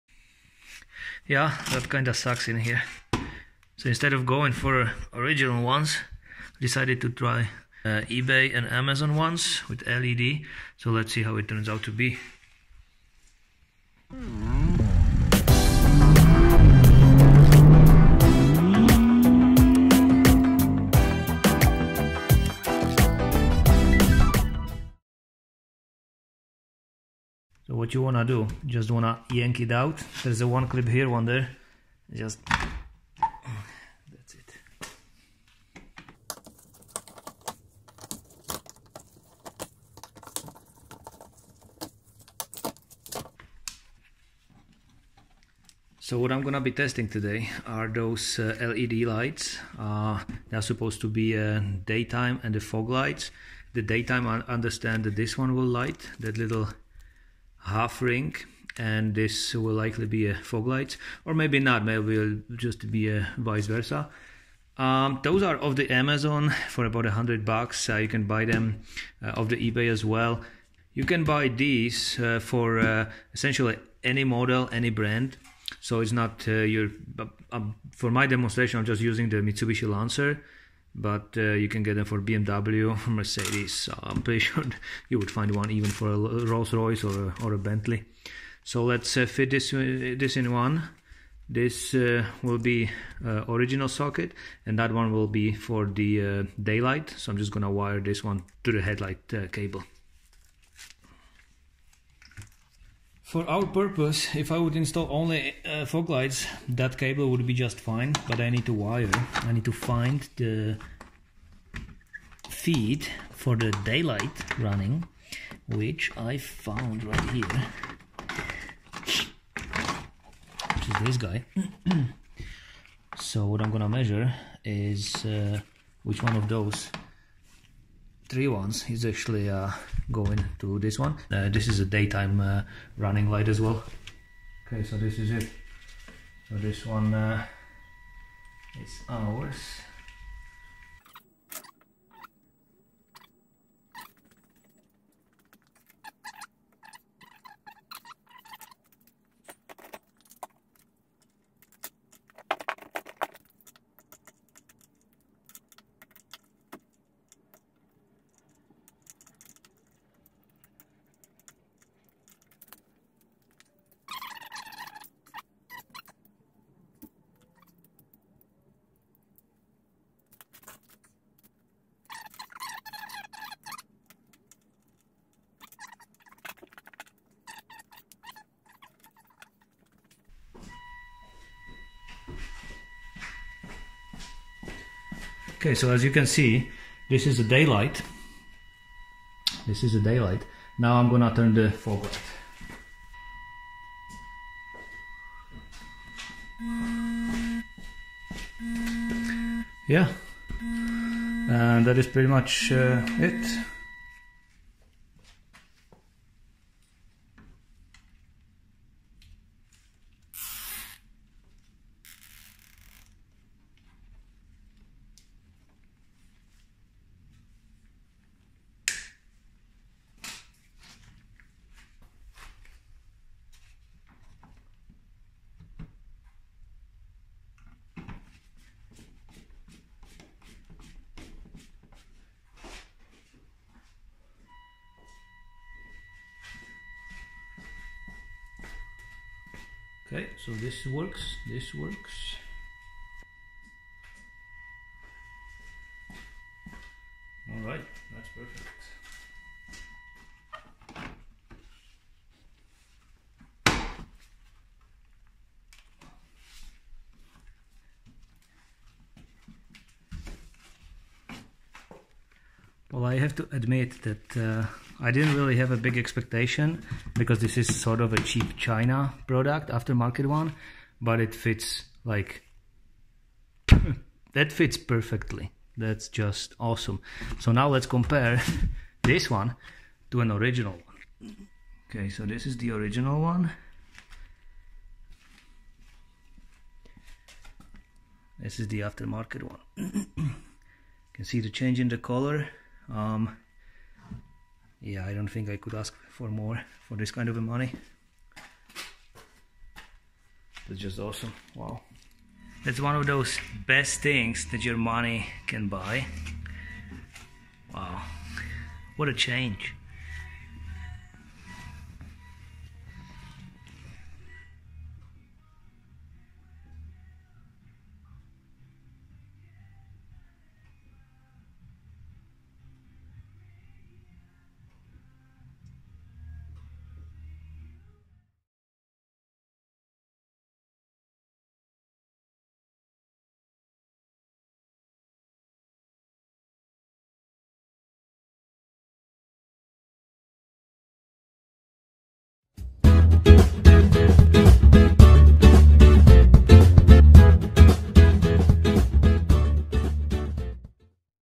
🧰 💵 Cheap eBay LED Fog Lights 🚨 | Worth the Money ? | Build Quality | Mitsubishi Evo
Are "cheap" Amazon, Aliexpress, Alibaba and eBay LED lights (in this case Fog assembly) better than Original.

In this Video - Direct comparison of Aftermarket VS Original and how to wire Daytime Running Lights intagrated into Fog ASSY. DIY

For my demonstration Im using 2018 Mitsubishi Lancer but the same lights are available for any brand and any model, wiring procedure will be always the same. Every decent auto parts store sells them also, but they all are made in china.

Links